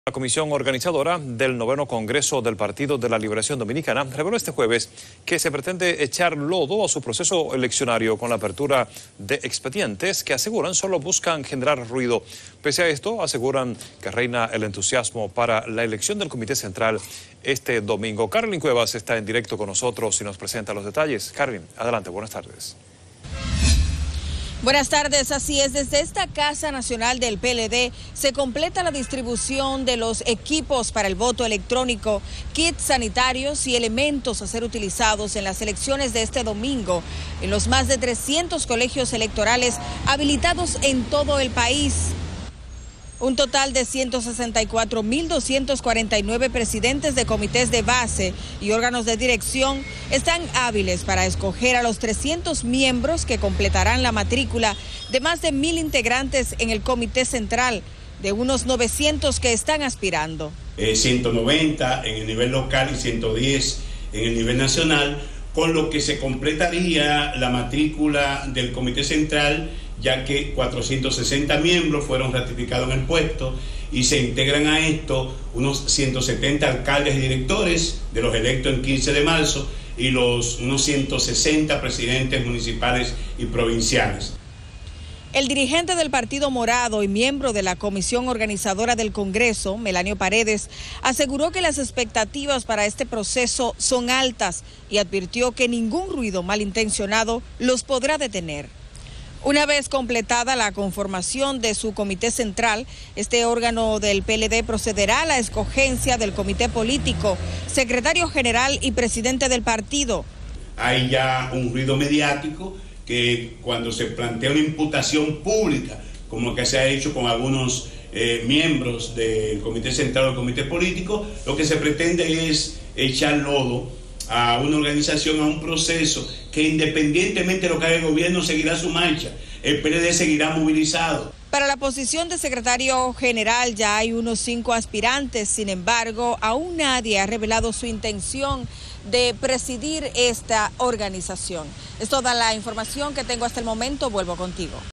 PLD ultima detalles elecciones comité central y advierte 'ruidos' no opacaran proceso
Desde esta casa nacional del PLD se completa la distribución de los equipos para voto electrónico, kits sanitario y demás elementos hacer utilizados este domingo en los más de 300 colegios electorales habilitados todo el país.

Un total de 164 mil 249 presidentes comités de base y órganos de dirección están hábiles para escoger a los 300 miembros que completarán la matrícula de más de mil integrantes al Comité Central, de unos 900 que están aspirando.